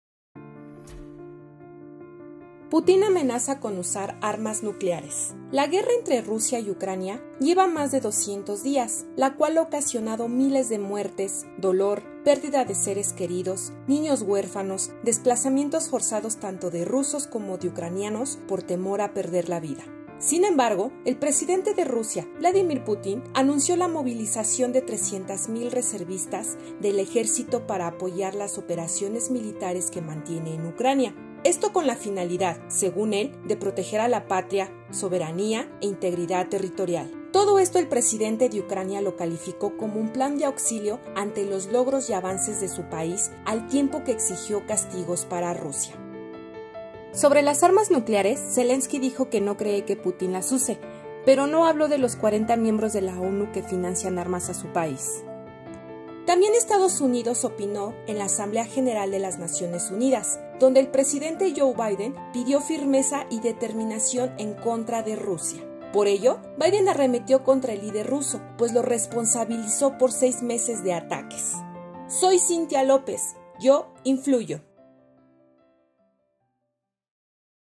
Putin amenaza con usar armas nucleares
La guerra entre Rusia y Ucrania no termina. Por ello, Putin no descarta la opción de usar armas nucleares.